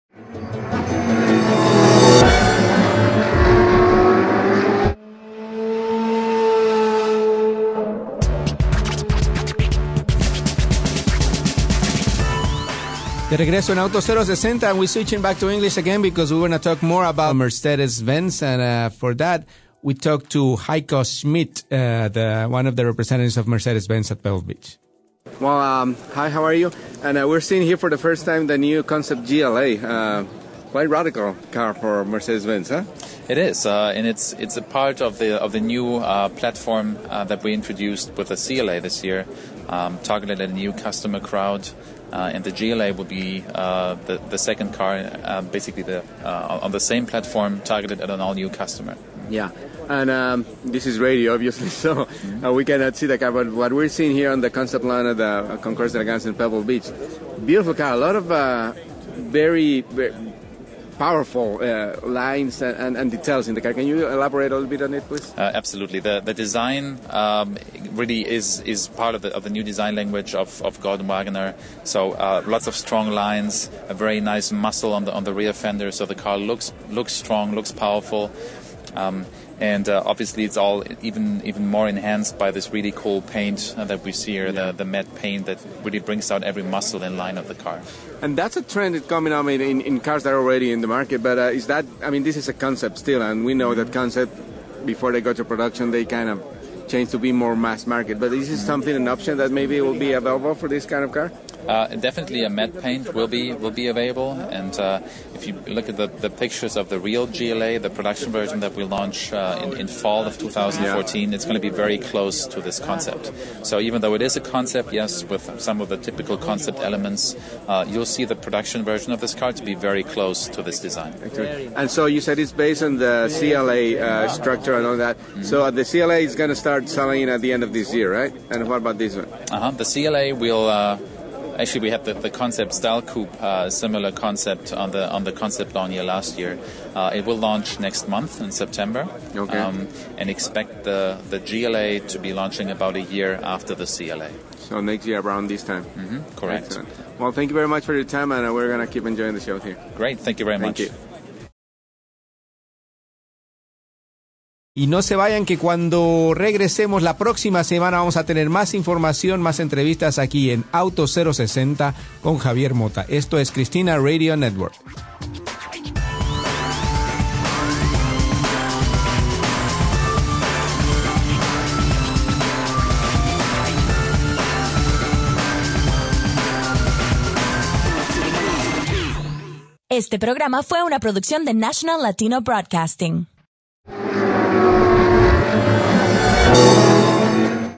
Autos 0-60 - Mercedes-Benz GLA Concept at Pebble Beach
An interview with Hiko Schmidt, Mercedes-Benz Department Manager for Compact Cars about the new GLA Concept which was on exhibit at Pebble Beach.

Aiming to set new benchmarks in efficiency, the new GLA mixes practicality with surefooted 4MATIC capability.

The first Mercedes-Benz in the fast-growing compact SUV segment is highly manoeuvrable around town (length x width x height 4,417 x 1,804 x 1,494 millimetres), lively on country roads and dynamic and efficient on the motorway (Cd figure 0.29). The high-quality appointments, developed with much love for detail, and the flexible interior clearly position the GLA as a compact premium SUV.